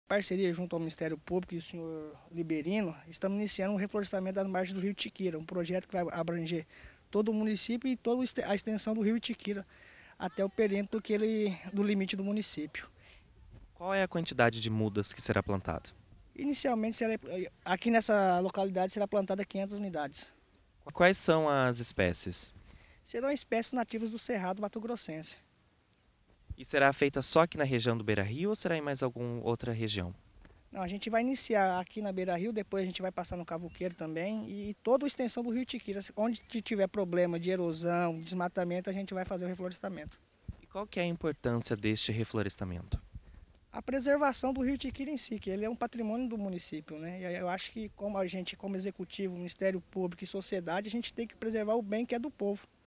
Margens do rio Itiquira recebe mudas de plantas nativas
RECORDTV ITIQUIRA CANAL 07